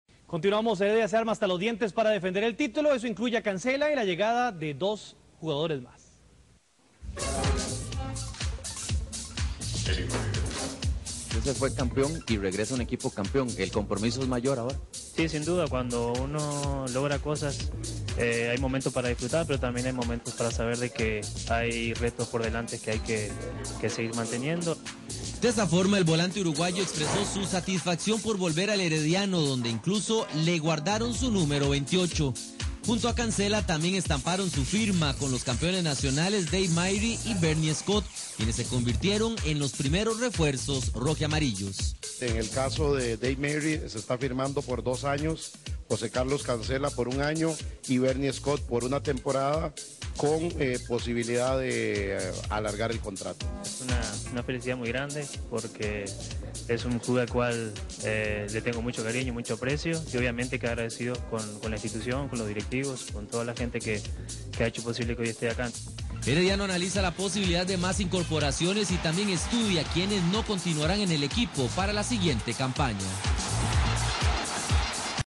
Heredia comenzó hacer sus contrataciones
Cancela,Myrie y Scott   son los nuevos refuerzos  cortesía teletica deportes